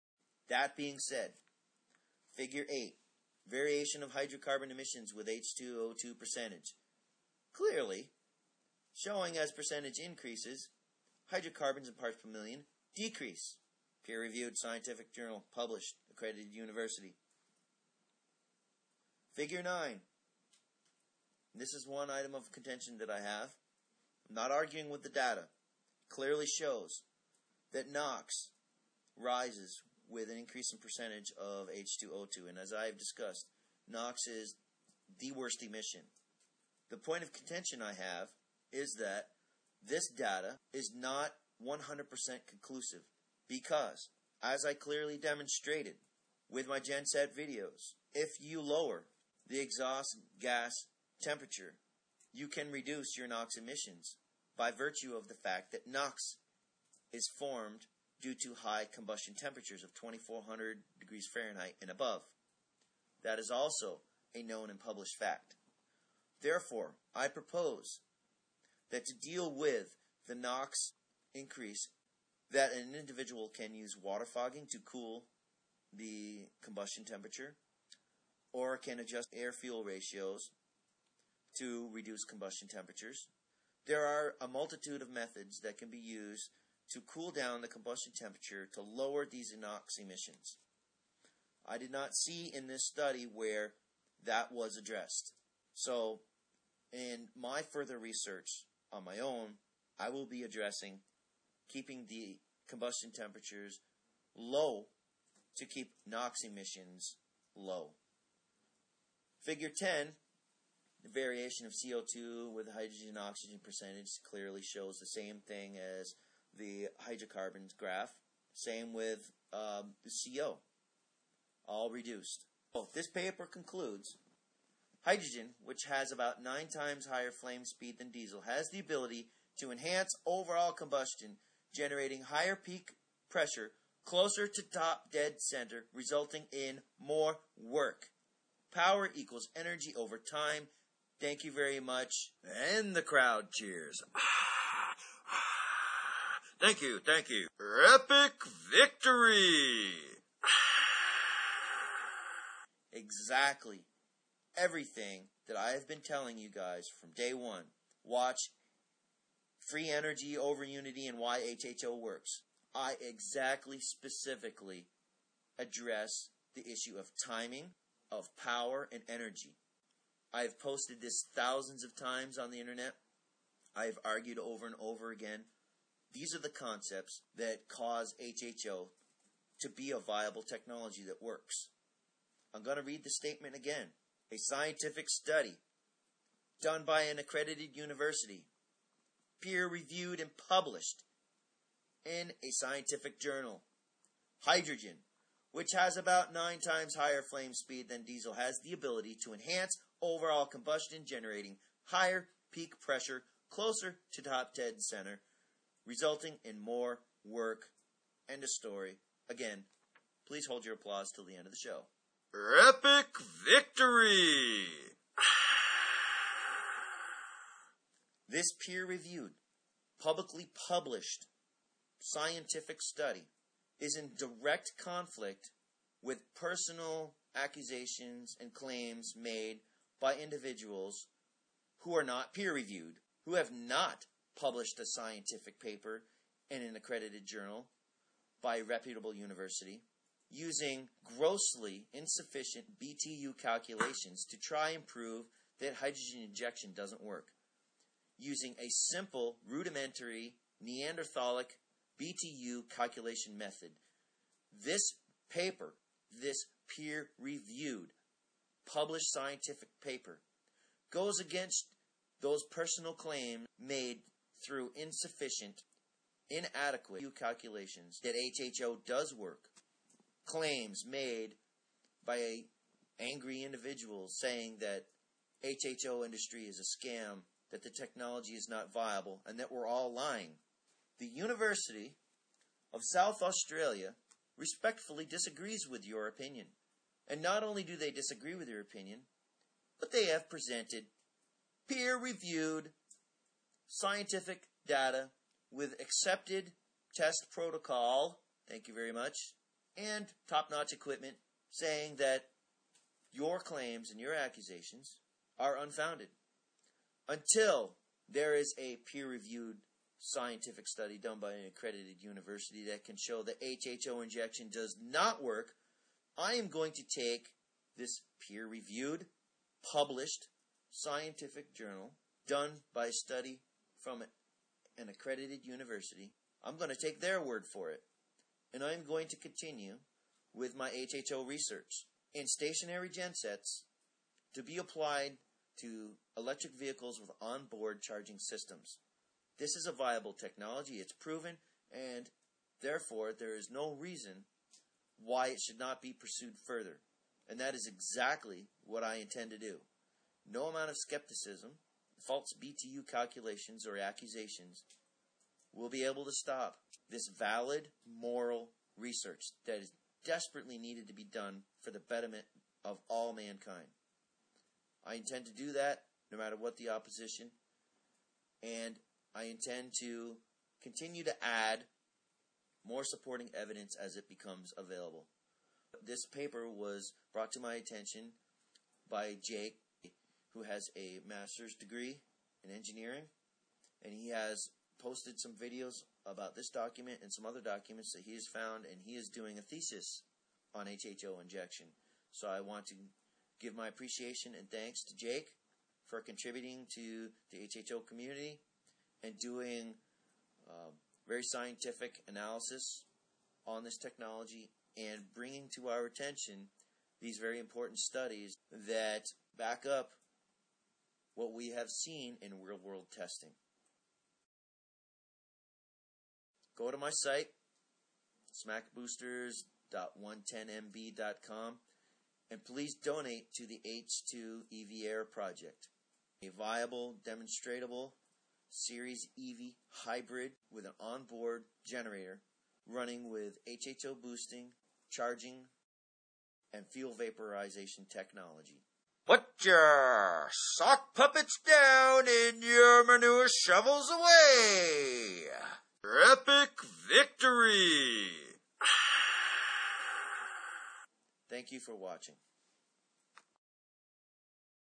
Scientific PROOF that HHO WORKS Part 3
This video is property of EletrikRide.
*** Mirrored Video From EletrikRide he writes,
You are NOT free to use any portion of this video sound, image, or otherwise without my prior consent.

No more comment wars or arguing.

I present to all of you loudmouthed, vile, vulgar, hate-consumed naysayers SCIENTIFIC, PUBLISHED, PEER REVIEWED DATA FROM AN ACCREDITED UNIVERSITY that PROVES not only that HHO works, but that the claims and statements I have made concerning this technology are TRUE.

Forget the mileage results that I have posted that prove HHO works.

Forget the genset tests that I posted that PROVE HHO works. Here is irrefutable documentation done in a similar manner on a diesel genset that is clearly documented using scientific equipment..

This published scientific study vindicates and absolves me from any and ALL false accusations made against me. This is epic victory against all the unjustified attacks made concerning my character.

I told you all this data would eventually be forthcoming. And this is only ONE of several papers published that support the viability of HHO technology. So much for those lacking BTU calculations. Here is more REAL WORLD data that establishes my dominance over the pontificating naysayer.

Break out the tissues, haters - your defeat is at hand.

VICTORY IS MINE!

Remember to please donate to my series EV hybrid project car with an HHO boosted on board genset range extender.